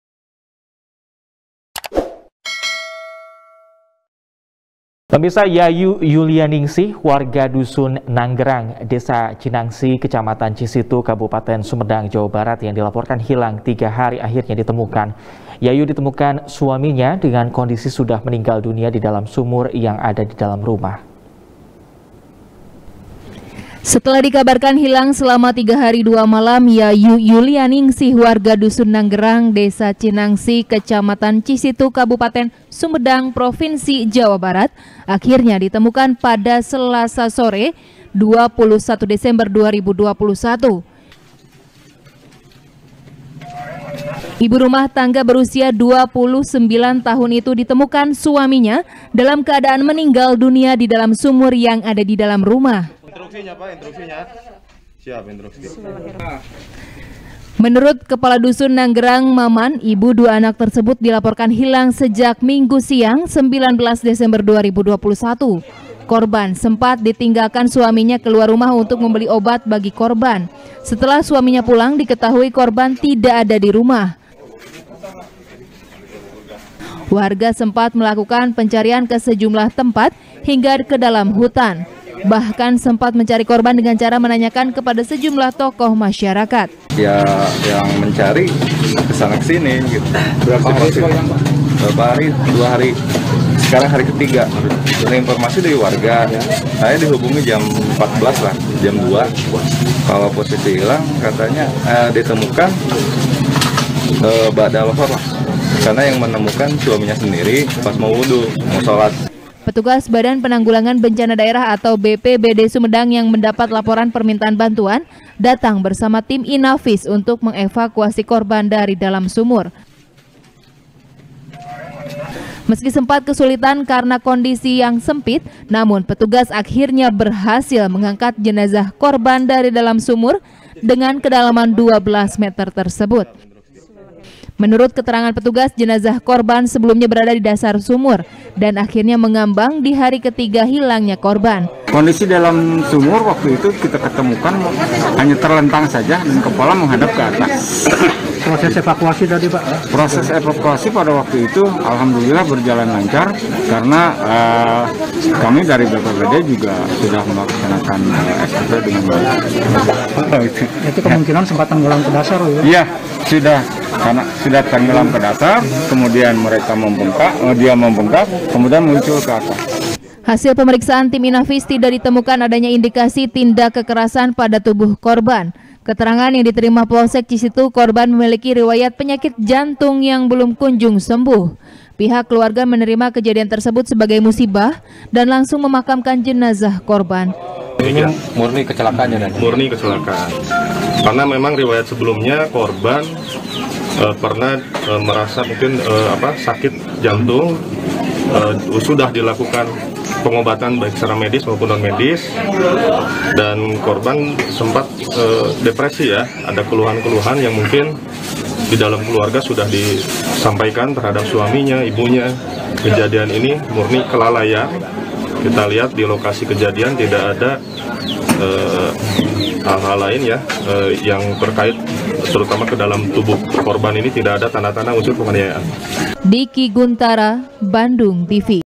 Hilang Tiga Hari, Jenazah Istri Ditemukan Di Sumur | SBR | BANDUNGTV
SBR | BANDUNGTV | IRFAN RAMANDA | Yayu Yulianingsih, Warga Dusun Nanggerang, Desa Cinangsi, Kecamatan Cisitu, Kabupaten Sumedang, Jawa Barat, Yang Dilaporkan Hilang Tiga Hari, Akhirnya Ditemukan. Yayu, Ditemukan Suaminya Dengan Kondisi Sudah Meninggal Dunia Di Dalam Sumur, Yang Ada Di Dalam Rumah.